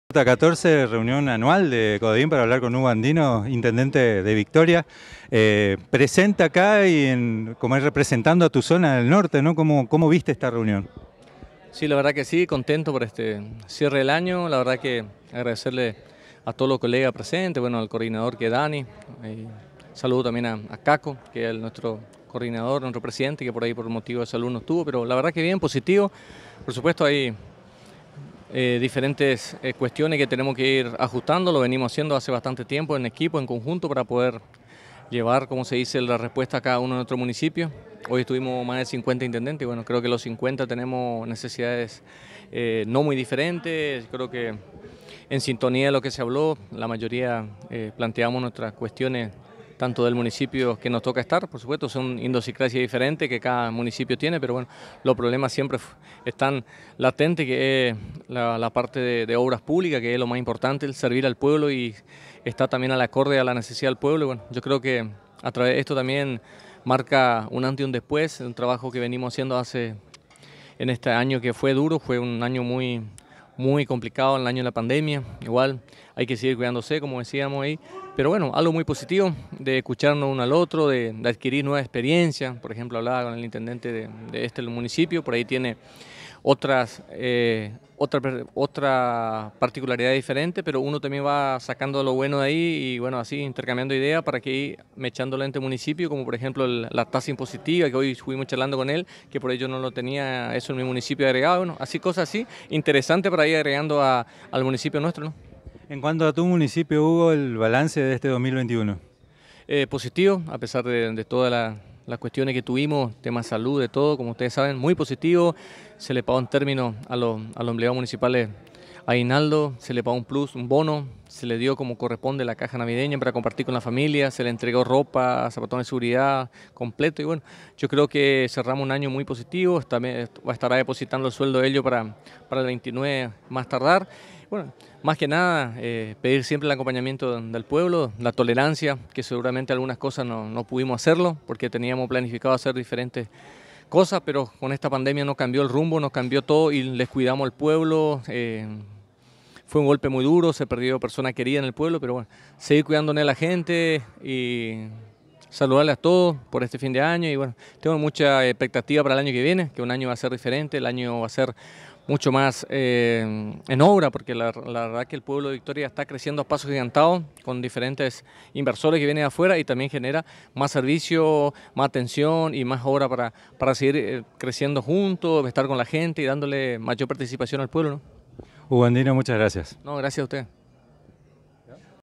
Hugo Andino - Intendente de Victoria.
Reunión Cierre de Año CODEIM con 50 Intendentes en Balneario "El Chapa". Colonia Alberdi. Lunes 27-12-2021